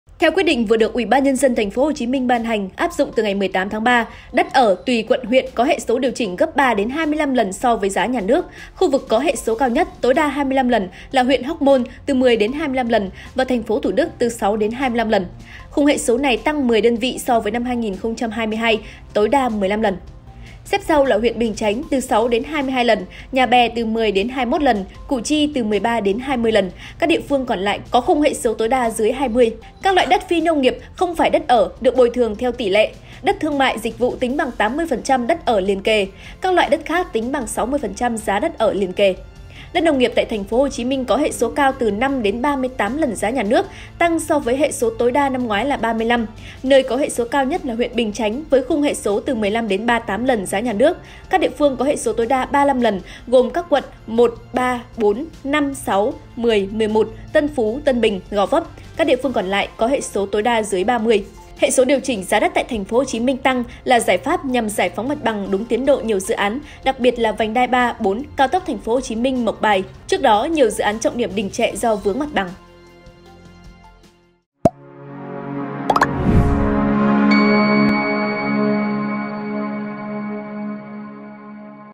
TP.HCM: Người dân được bồi thường tối đa gấp 25 lần giá đất Nhà nước | Truyền hình Quốc Hội Việt Nam
Truyền hình Quốc Hội Việt Nam | Theo quyết định vừa được UBND TP.HCM ban hành, áp dụng từ 18/3, đất ở tuỳ quận huyện có hệ số điều chỉnh gấp 3-25 lần so với giá nhà nước. Khu vực có hệ số cao nhất, tối đa 25 lần, là huyện Hóc Môn (10-25 lần) và thành phố Thủ Đức (6-25 lần). Khung hệ số này tăng 10 đơn vị so với năm 2022 (tối đa 15 lần).